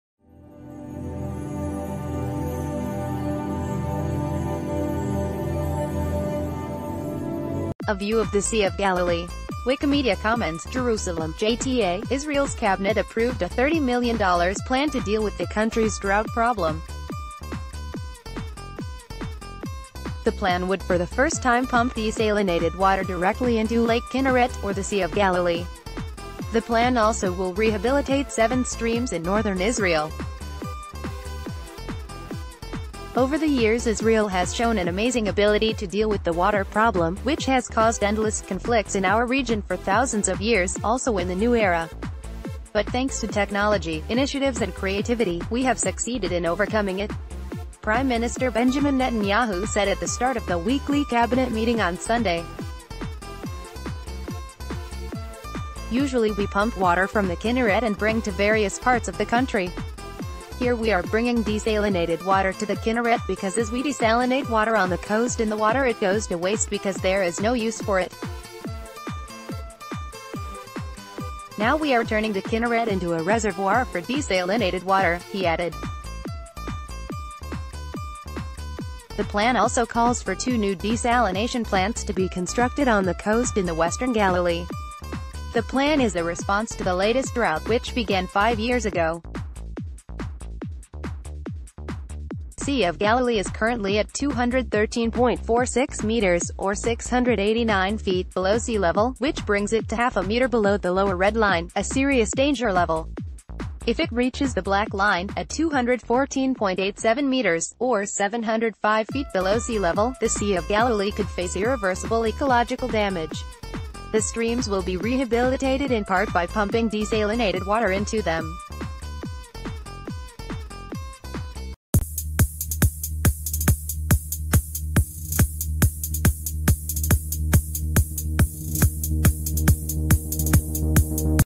Breaking News | Israel approves $30 million plan to counter 5-year drought
A view of the Sea of Galilee. (Wikimedia Commons) JERUSALEM (JTA) — Israel’s Cabinet approved a $30 million plan to deal with the country’s drought problem. The plan would for the first time pump desalinated water directly into Lake Kinneret, or the Sea of Galilee. The plan also will rehabilitate seven streams in northern Israel. “Over the years Israel has shown an amazing ability to deal with the water problem, which has caused endless conflicts in our region for thousands of years, also in the...